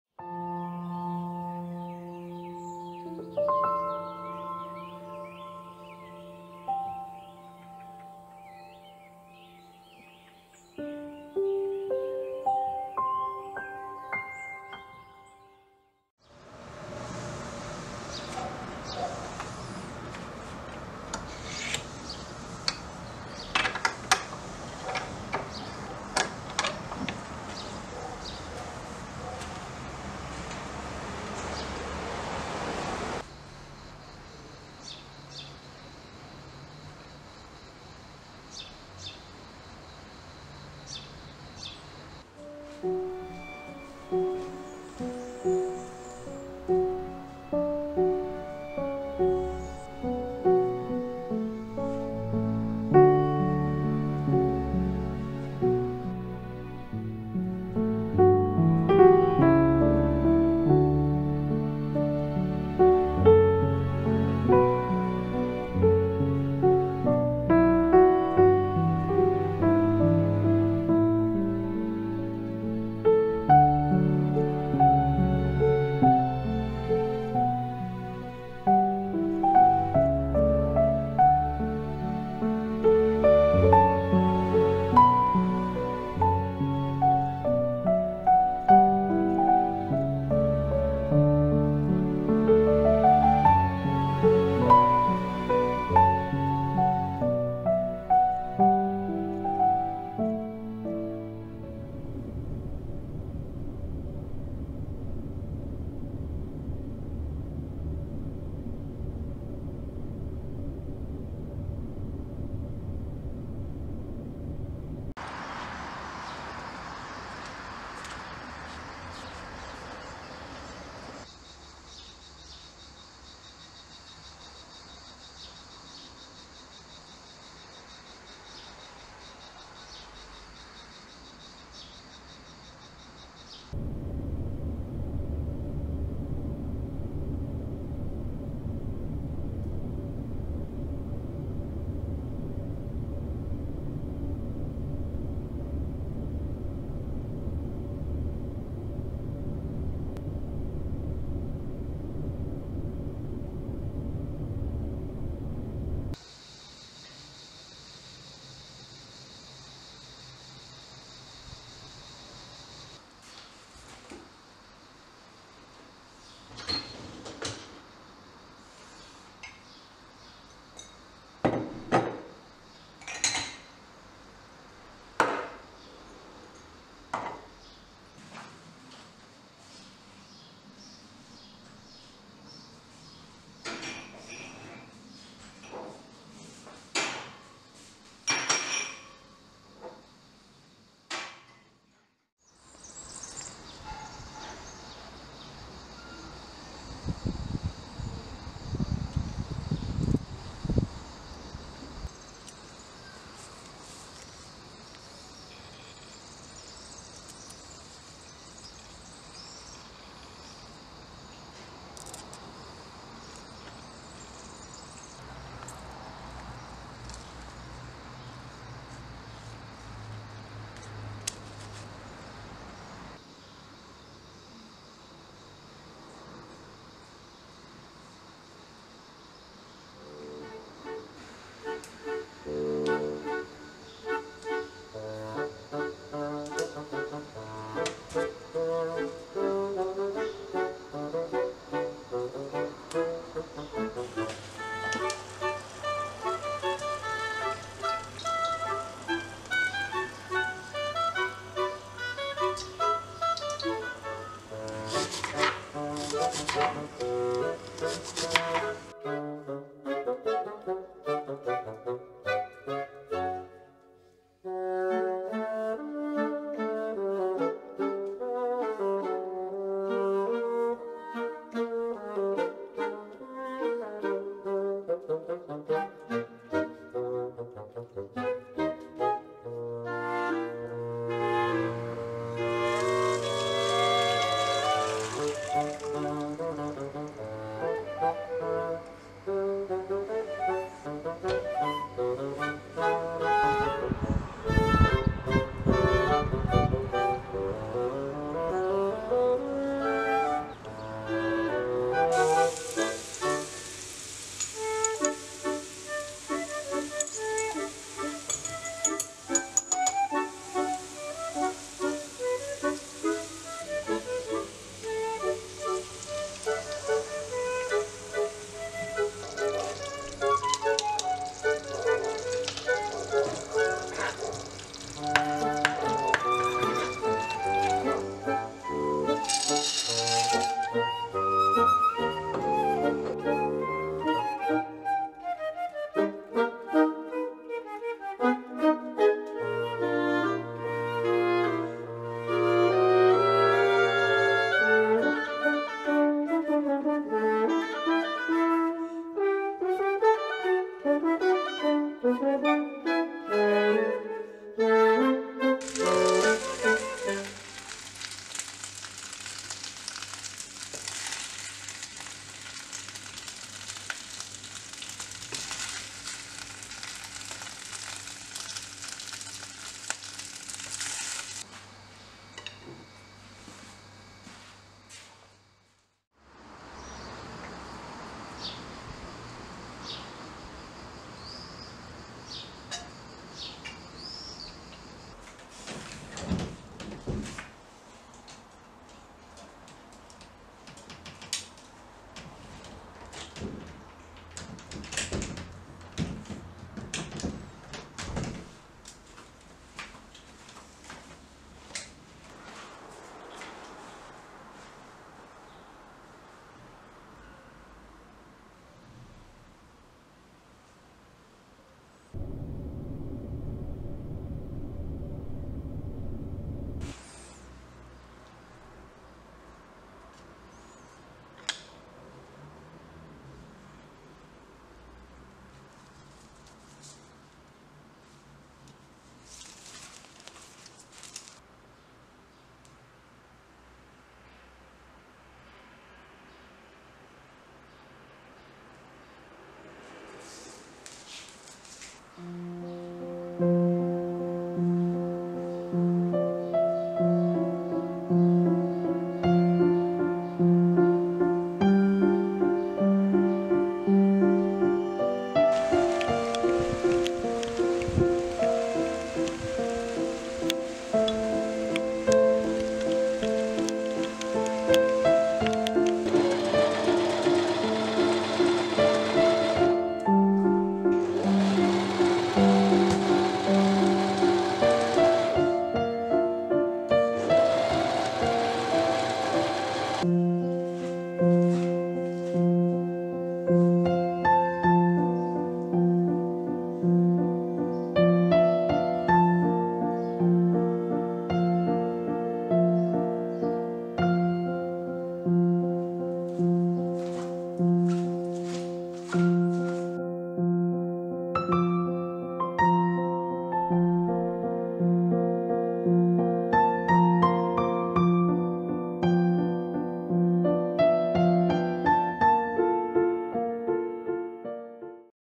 A day with me💐- a sweet breakfast, picking aromatic herbs, wild fennel sauce, a little bit of sewing
A peaceful day with me, I hope this video will be of your liking my friend :)